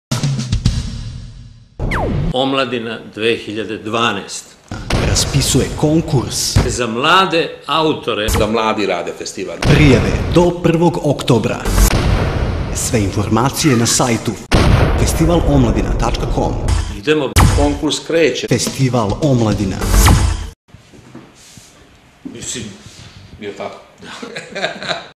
Omladina konkurs reklama Bata Kovac i Gabor Lengyel
Kornelije Kovac i Gabor Lengyel pozivaju Vas na KONKURS Festivala Omladina 2012 ! Prijave do 1. oktobra !! Sve informacije na sajtu